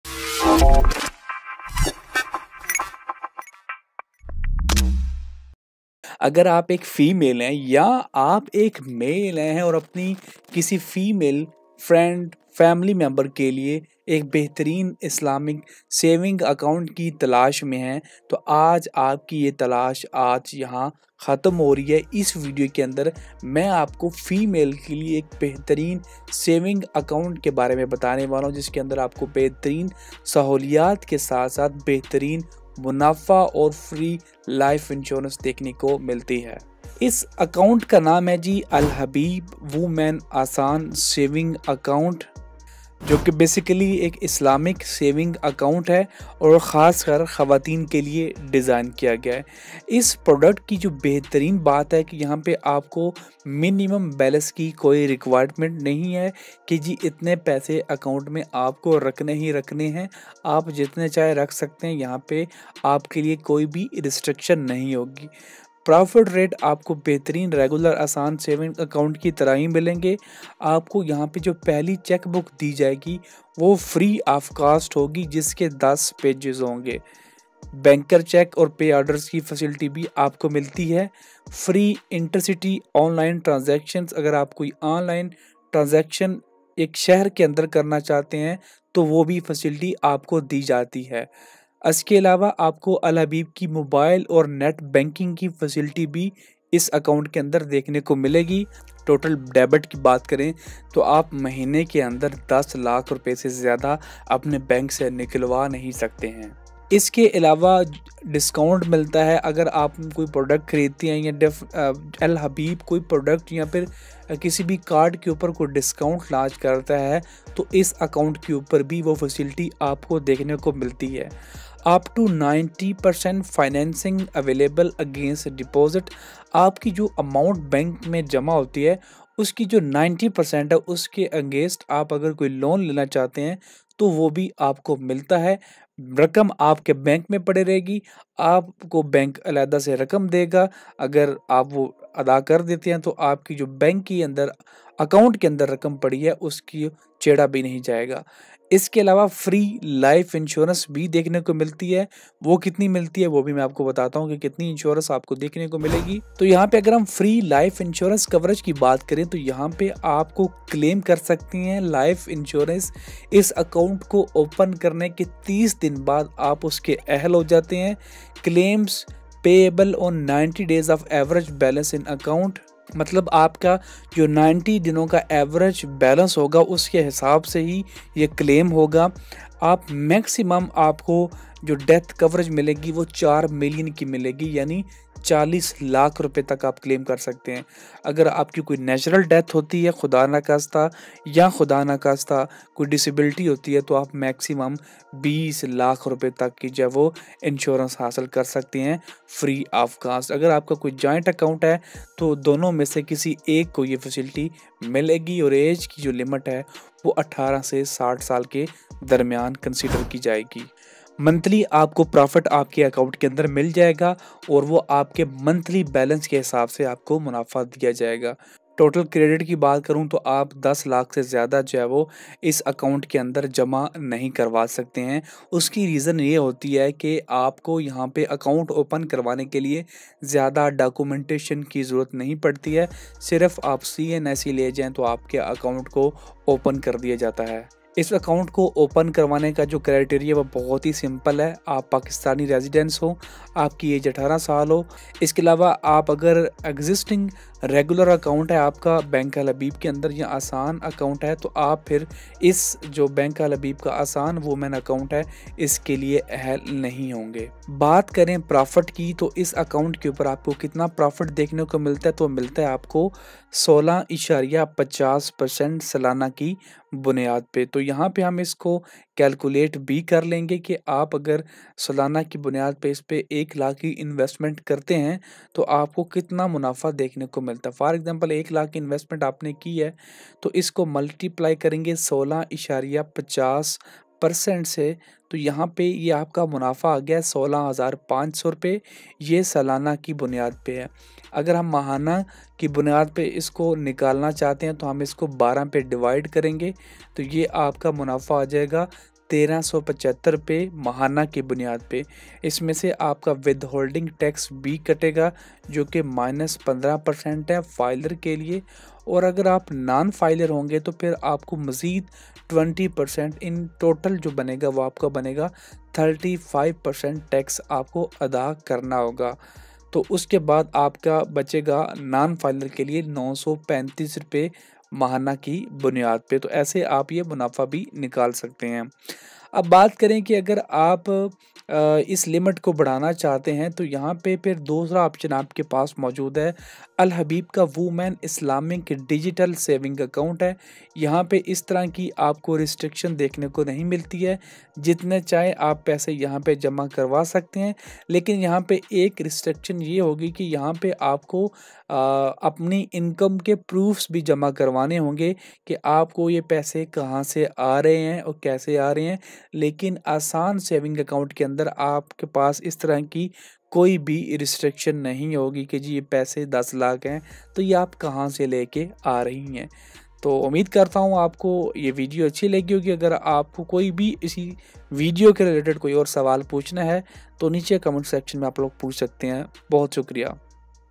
AL Habib Woman Asaan Saving Account 2024 | Best Saving Account for Females in Pakistan
AL Habib Woman Asaan Saving Account is specifically for those who are looking to save with minimal requirements. Availing financial services is not challenging anymore as this account is an easy and reliable solution for women to grow their savings and enjoy several benefits.

City 41
Elction Box
Business Chowk
Helan MTM Box
Q tech
Grow With Haider
Bank News